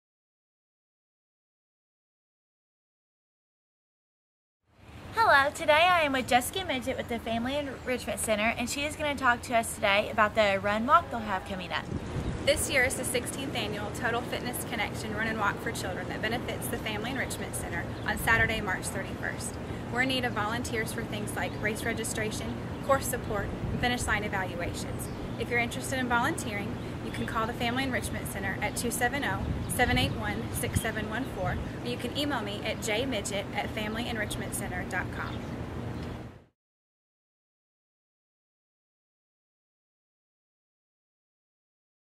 Alive Video (3)
Third video of the "Be ALIVE" Video series promoting volunteerism throughout the Bowling Green community from the ALIVE Center. Featuring the Family Enrichment Center.